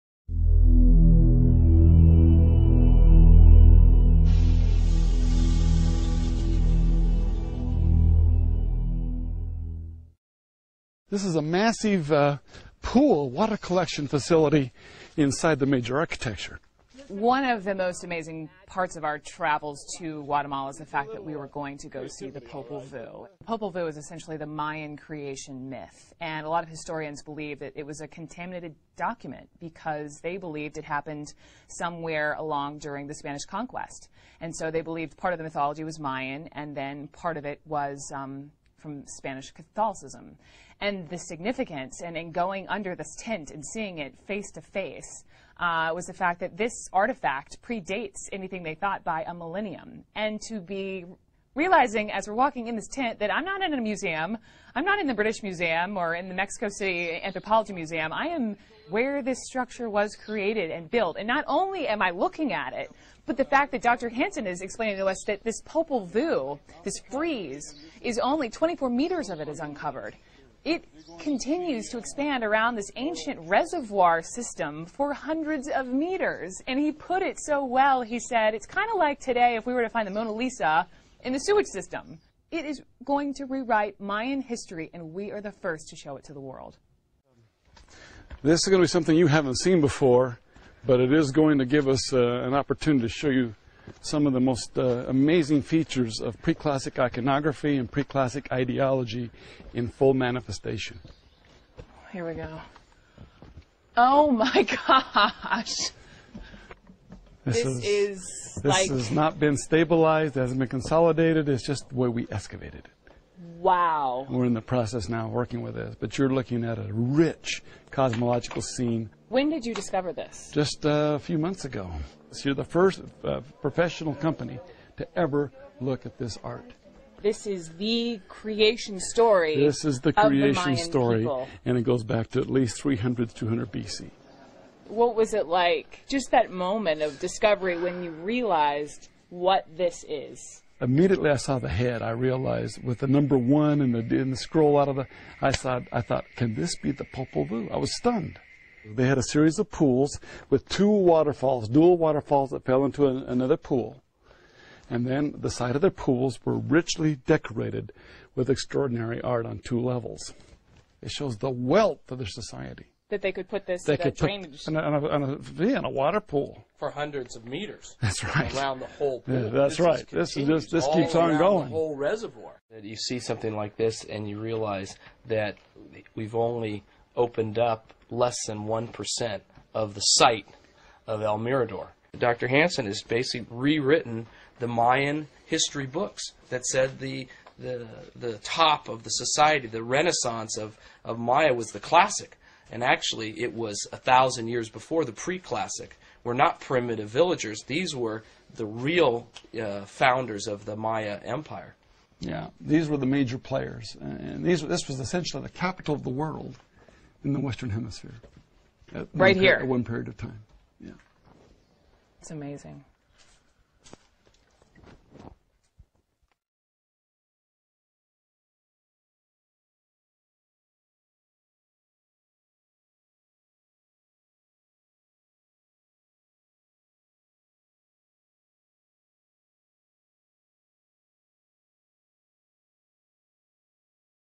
Popol Vuh - The Mayan Story of Creation
How this compares to the creation story in Genesis. So many similarities but it is a diluted version of what is truth.